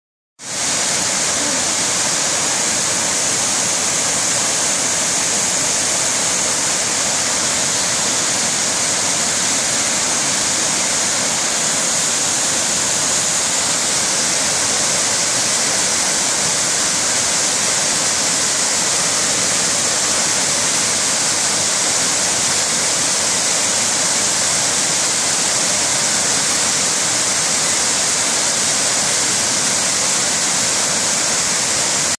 Lower Long Creek Falls
The lower falls on a "goat trail" off the AT-BMT just below the main Long Creek Falls.